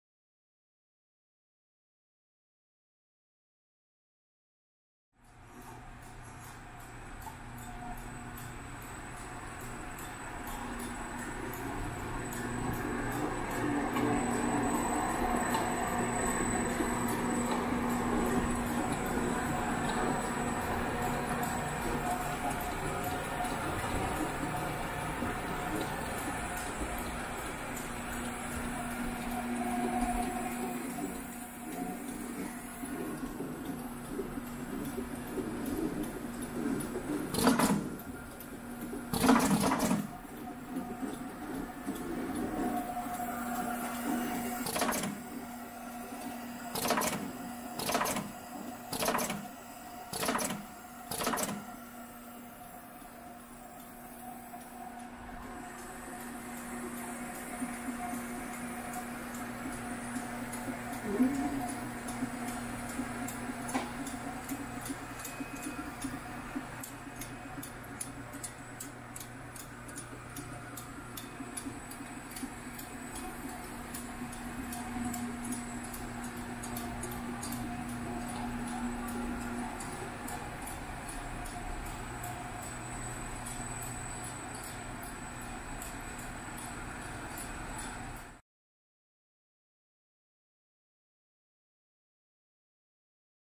Axon EZ-130 Shrink Sleeve Labeler and Heat Tunnel SIGMA Equipment
This is a functional video of an Axon EZ-130 Shrink Sleeve Labeler and Heat Tunnel. for more available equipment.